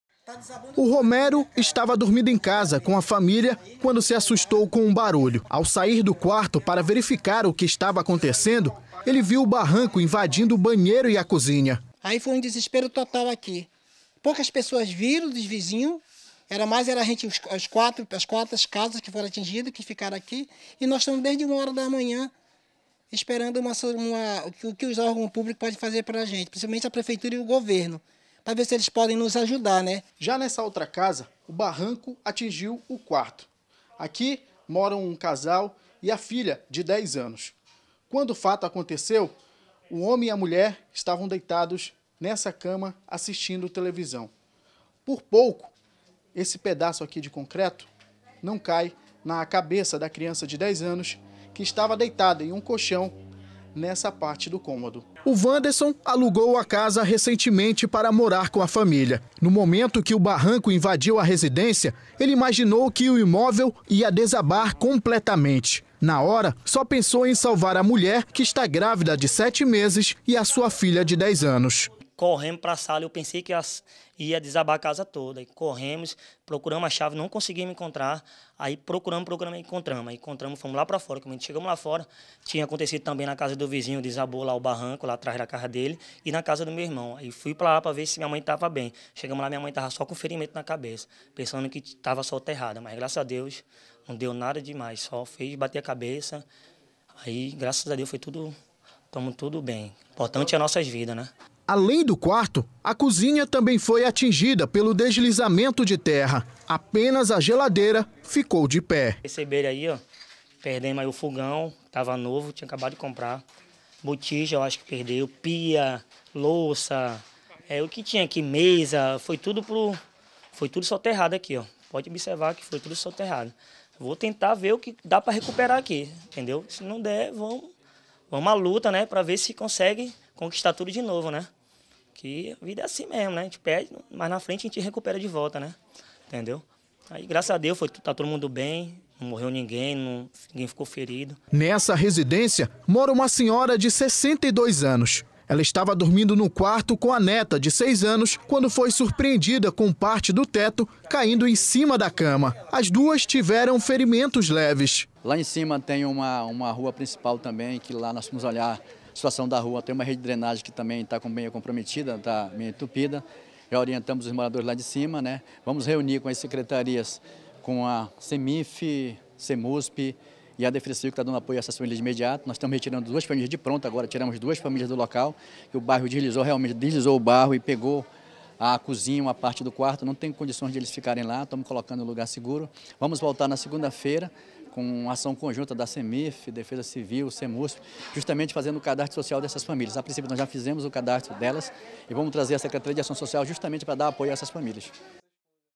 DESABAMENTO EM MANAUS
Três casas e uma igreja foram atingidas por um barranco que deslizou na madrugada deste domingo, 24, no conjunto Boas Novas, no bairro Cidade Nova, Zona Norte de Manaus.